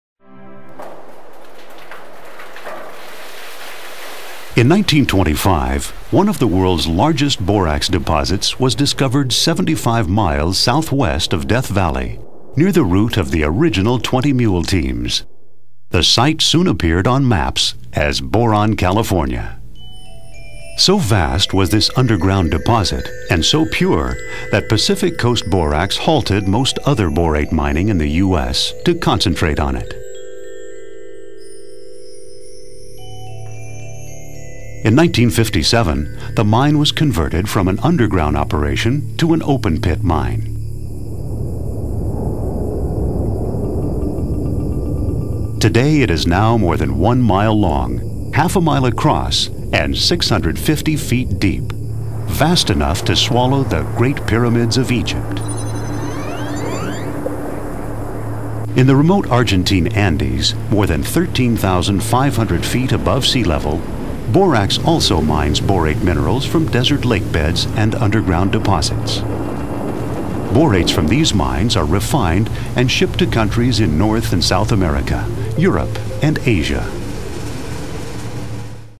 Borax Mine
In 1925 Pacific Coast Borax officially began  mining at a place near Kramer Junction in the Mojave Desert. The town that grew around the mine would be called Boron. Just 31 years later the mine would go from underground mining to an open pit. Today the mine operated by Borax a division of Rio Tinto, is the largest open pit mine in the state of California. The Borax from here goes around the world and is in thousands of things like computer monitors, televisions, fiberglass, pyrex, insulation, disinfectants and hundreds of other uses including 20 Mule Team Borax laundry additive.